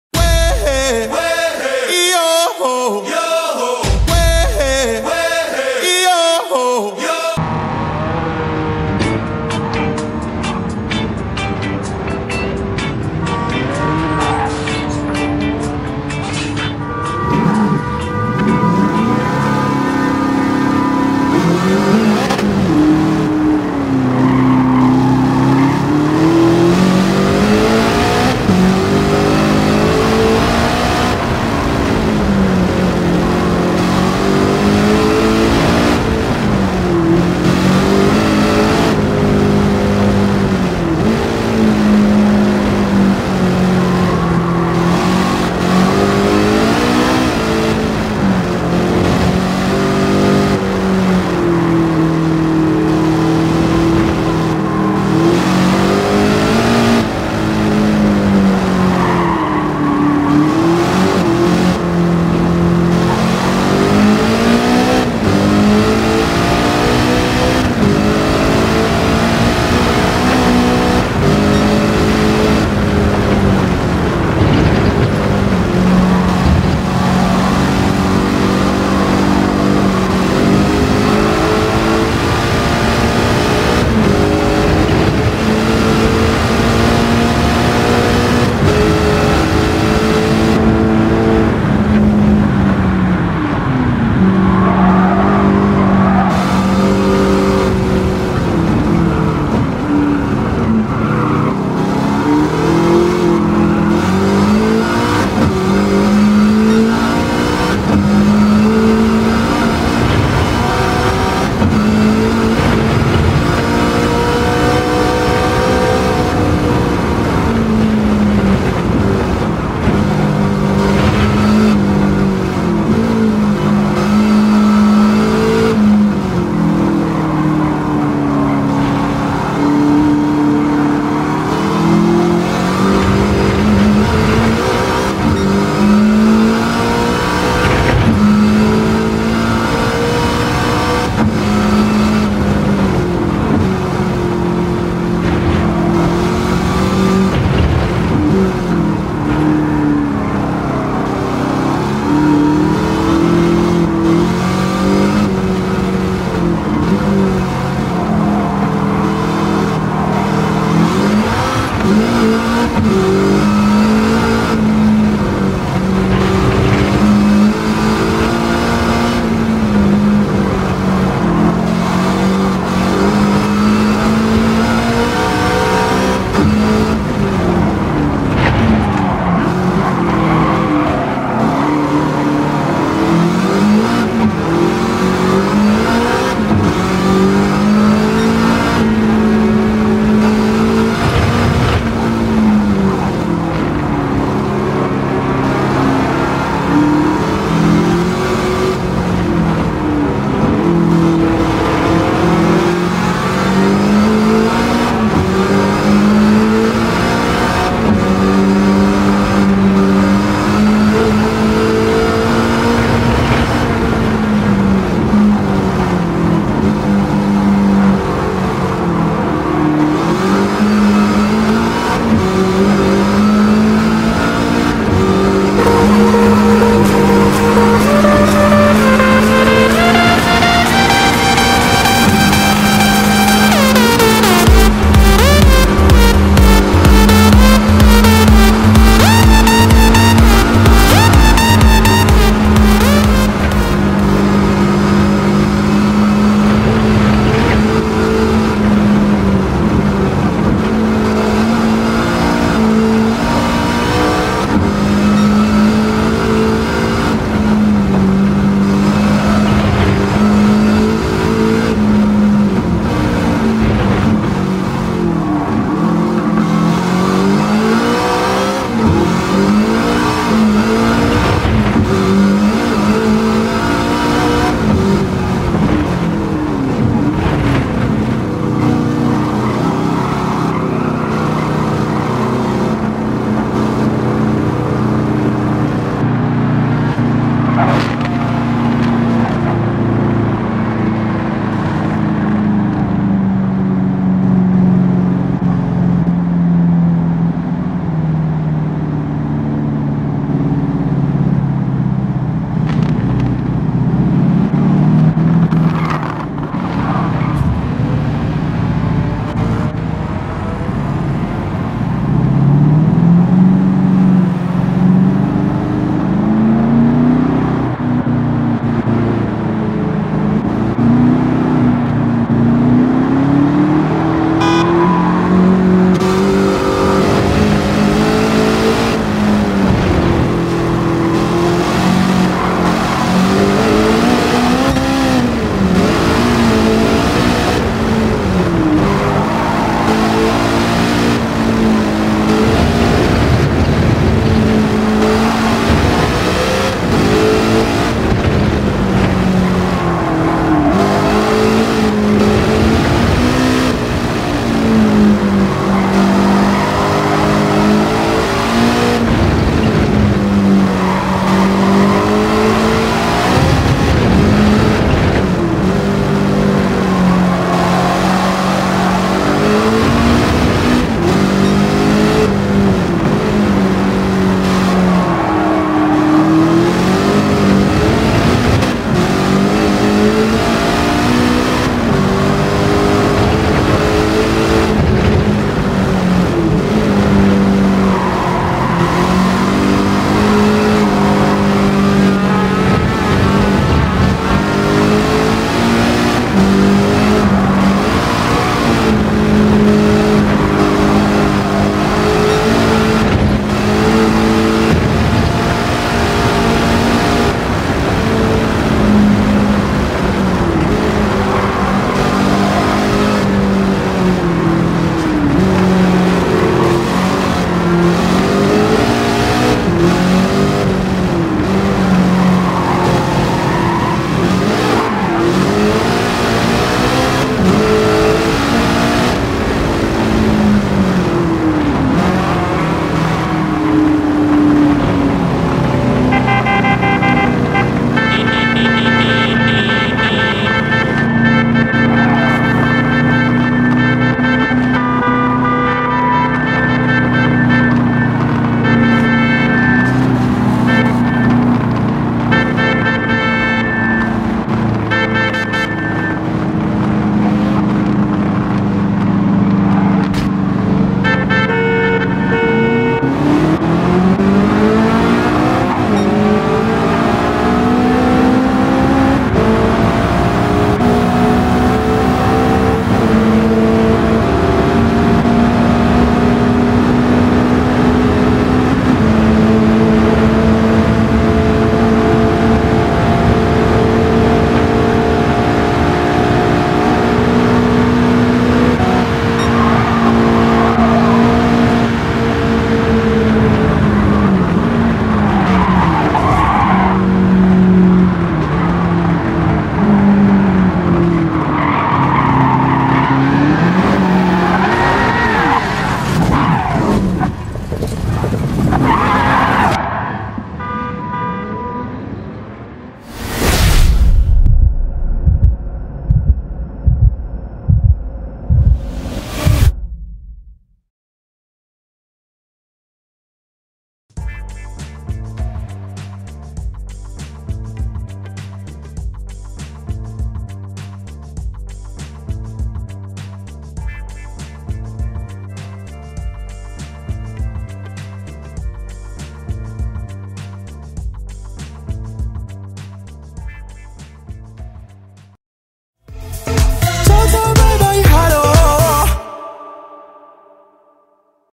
Un giro all'Inferno...verde! Lobby 15 EP. 4
Questa è la quindicesima lobby, organizzata con i miei iscritti! L'alta partecipazione (numero di partecipanti doppio rispetto alla prima) ha reso il tutto molto interessante! Buona visione